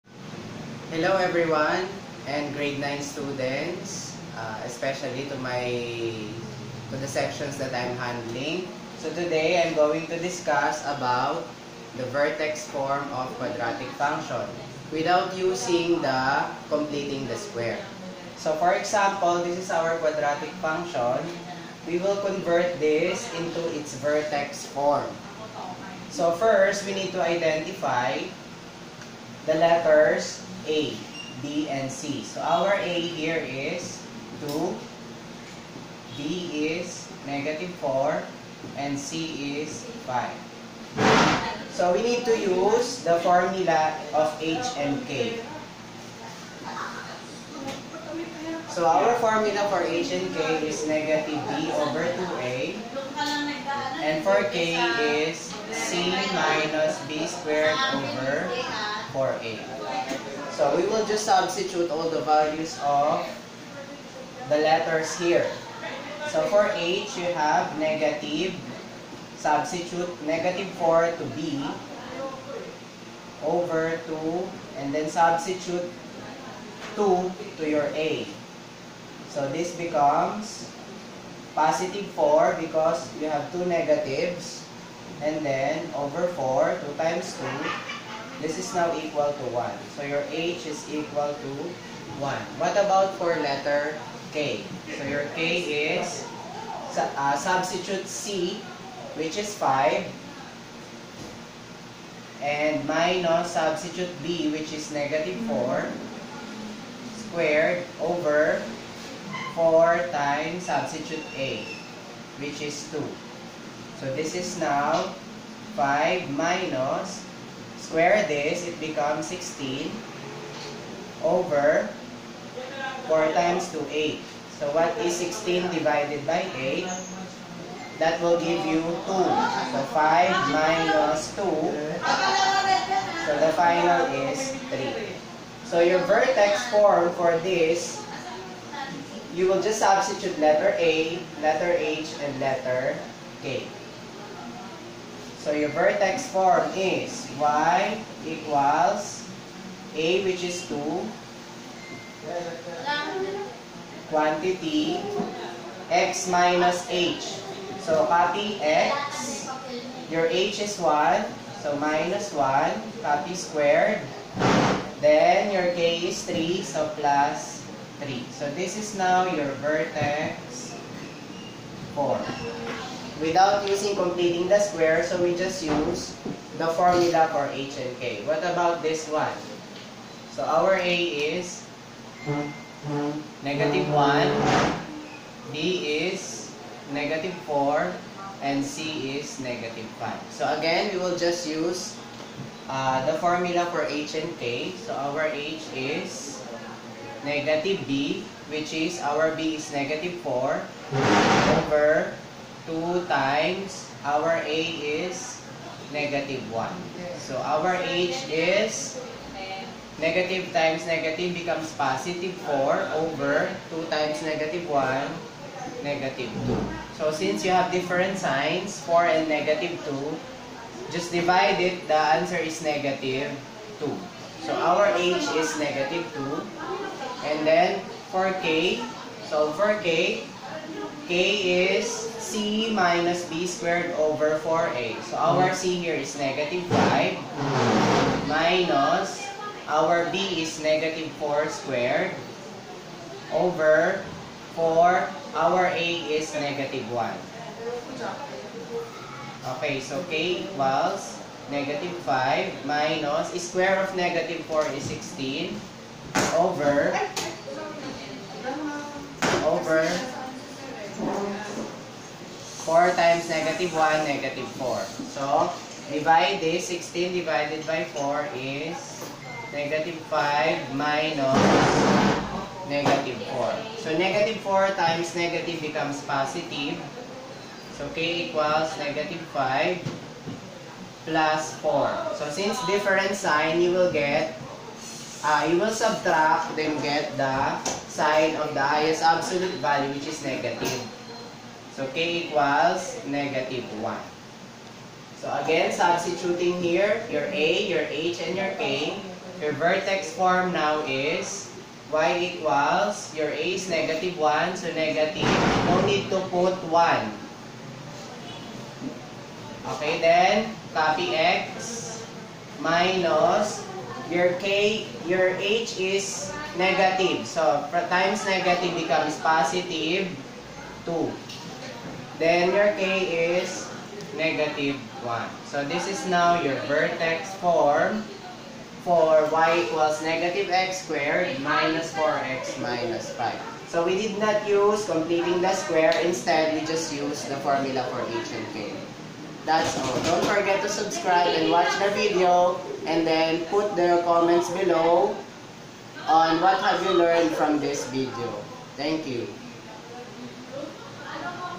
Quadratic Function in Standard Form to It's Vertex Form
Grade 9 Students
This will be considered your Performance Task!
Comments will be checked.

This is for my Grade 9 students, but if it is useful and helpful for you, you may also watch it and learn from it.